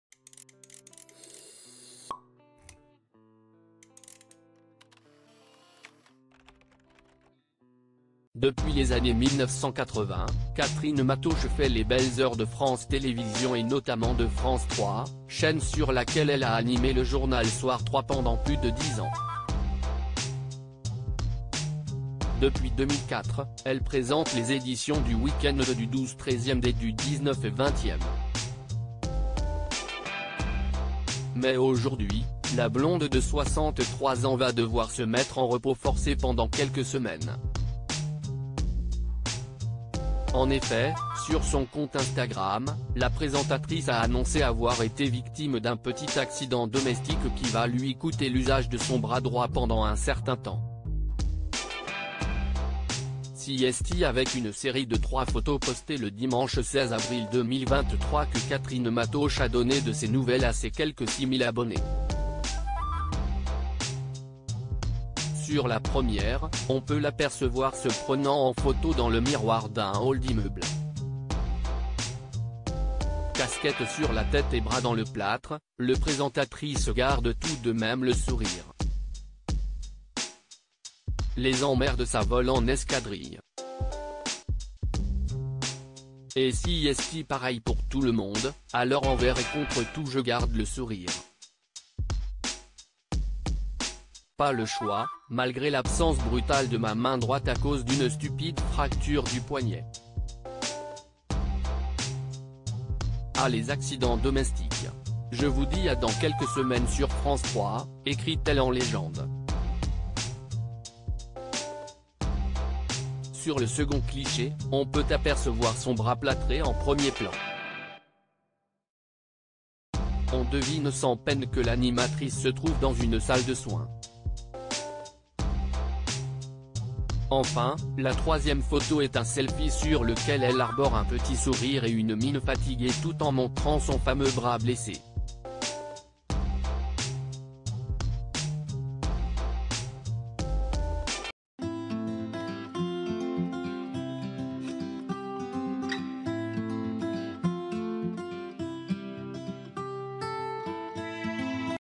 "Les emmerdes, ça vole..." : Catherine Matausch absente de France 3, elle révèle s'être blessée lour
"Les emmerdes, ça vole..." : Catherine Matausch absente de France 3, elle révèle s'être blessée lourdement...